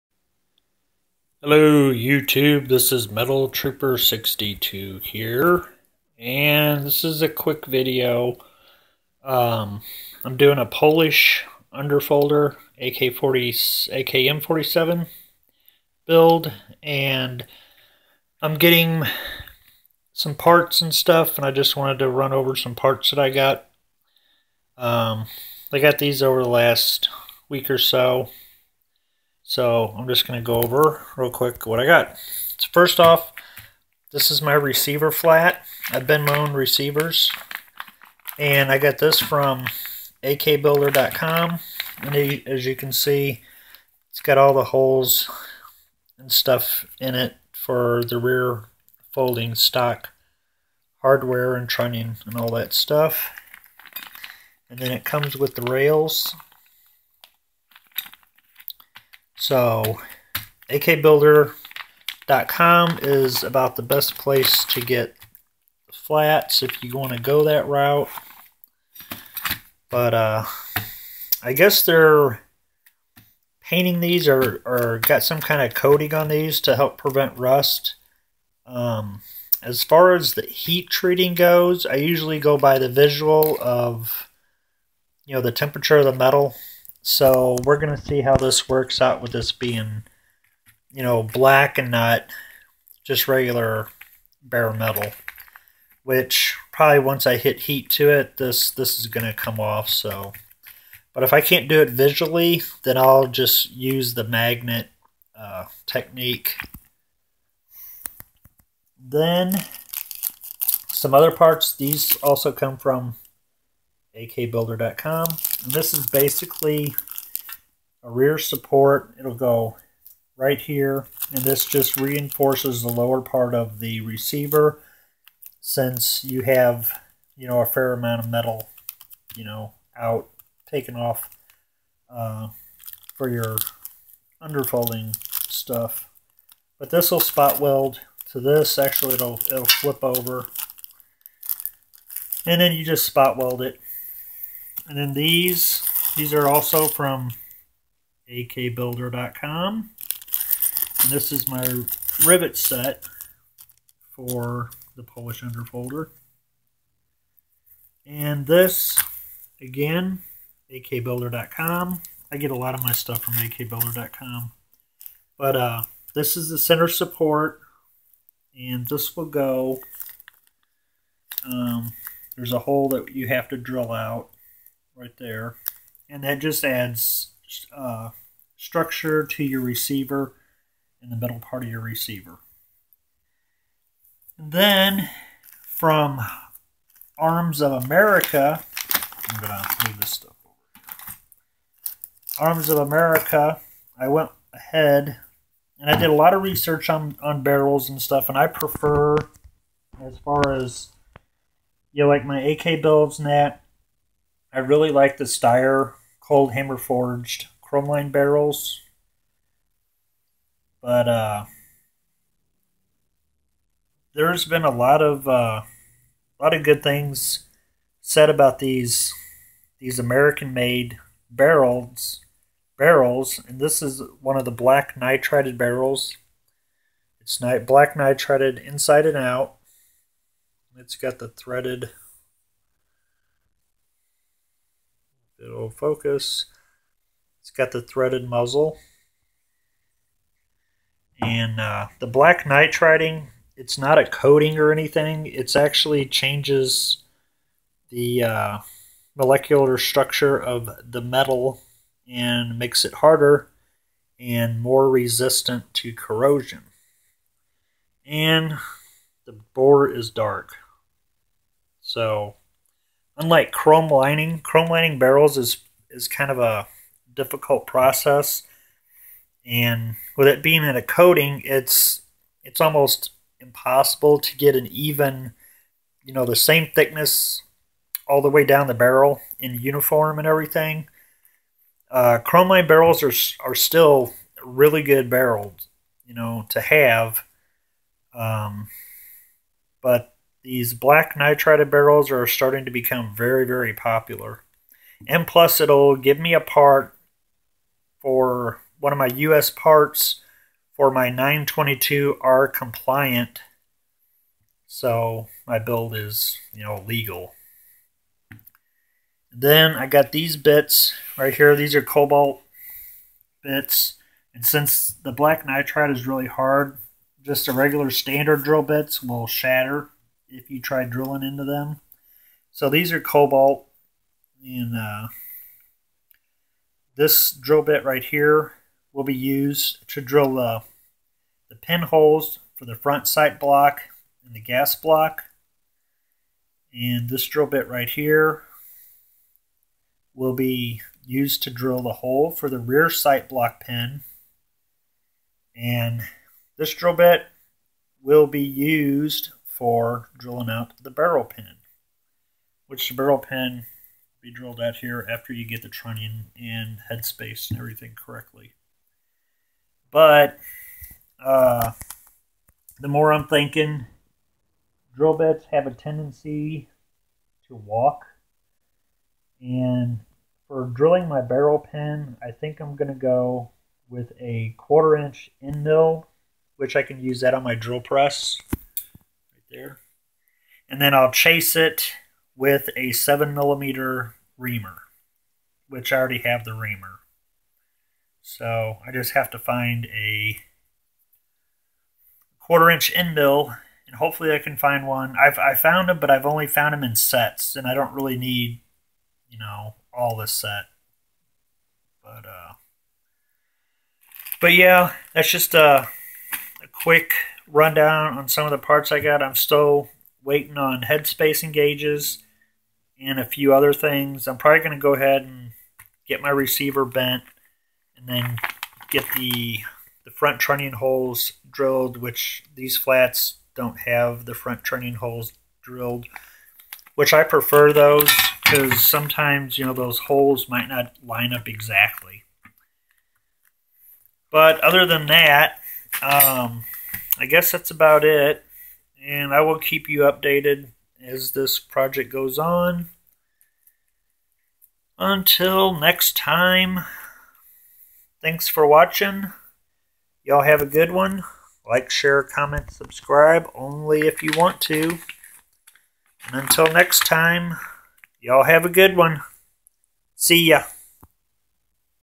Polish Underfolder AKMS Build Receiver Flat and Barrel
Hi everyone. This video shows what I'll be using for my future Polish Underfolder AKMS build. I bought the receiver flat, rivet set and 1mm center support from AK Builder.com. The barrel and cobalt drill bit set came from Arms of America the same place I bought the parts kit from. These are 2 awesome companies for your AK build needs. Great prices and awesome customer service.